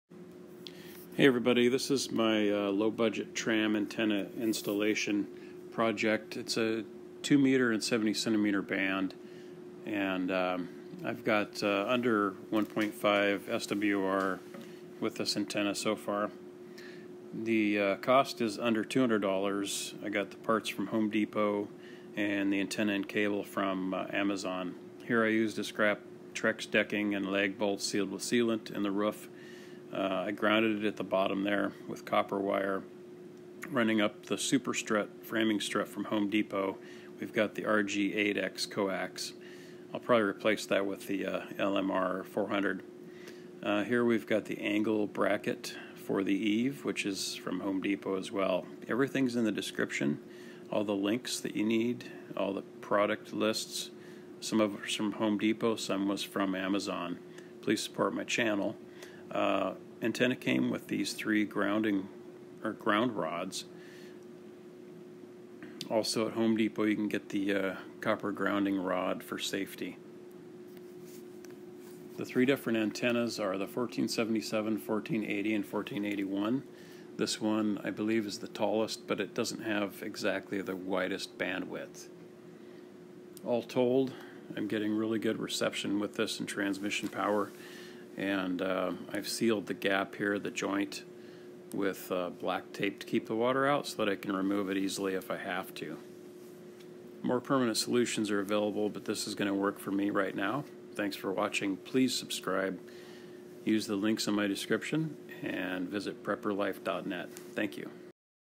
Under $200 Ham Radio Antenna Install (Tram 1477, 1480, 1481)
This a Tram 1480 Dual Band 2 meter & 70cm Amateur Radio antenna installation video. Applies to Tram 1477 & Tram 1481 as well.  Antennas: Tram 1477 Dual Band
Tram 1481 Dual Band
Tram 1480 Dual Band (used in video)
I am getting under 1.5 SWR with a Baofeng UV-5R handheld radio with a Tram 1480.  Note you can monitor 2m & 70cm Amateur Radio - but to transmit - you have to have a Tech License from the FCC...  Anyway, here are the parts you need.

Mast & Cable/Coax Parts List.
On Amazon-
Handheld Baofeng UV-5R Ham Radio:
Coax:
LMR400 (best for over 50')
RG8X (good for up to 50')
At Home Depot-
Mast:  Superstrut 12GA 10' Metal Framing Strut (just search HD website)-
Superstrut 5 pack Bolts
Superstrut 10 pack nuts
Lagbolts (size depends on your application)
Eave Bracket:  YardGard 2-3/8 Wood Adapter (for fencing)
Roof Sealant (your choice)
Copper Grounding Rod
6-8ga Copper grounding wire.
NOTES: ** Tram 1477 specs, 4ft tall; range 144-148mhz/430-460mhz; 3.5gain/6gain.  150watts max.
**Tram 1480 specs, 8ft tall; range 144-148mhz/440-450mhz (works fine up to 469mhz); 440-450mhz; 6gain/8gain. 200watts max.**Tram 1481 specs, 17ft tall; 144-148mhz/430-450mhz; 8gain/11gain.  THE MHz ranges are from the manufacturer as to what they are tuned for.  However, these will work beyond that range.  I have been able to get 1.1 SWR on 469MHz with the 1480 using LMR400 coax.
I do recommend going higher and using LMR400 cable.  Structurally, I'd need more bracing to go higher.  Grounding is a consideration as well as lightning protection.  Never leave a radio connected to antenna during a storm.  Follow all safety recommendations from ARRL & Local codes.